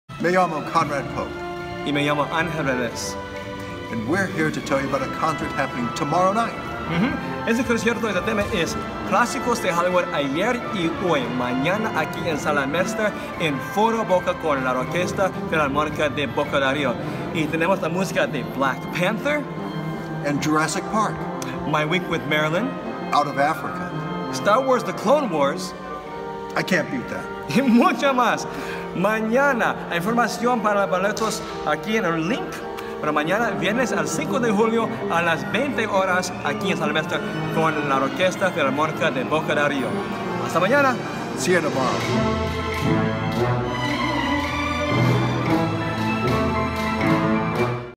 Hollywood Classics: Old and New promo video
Promotional video for Hollywood Classics: Old and New (Clásicos de Hollywood: Ayer y Hoy) with the Orquesta Filarmónica de Boca del Río área Foro Boca in Veracruz, Mexico.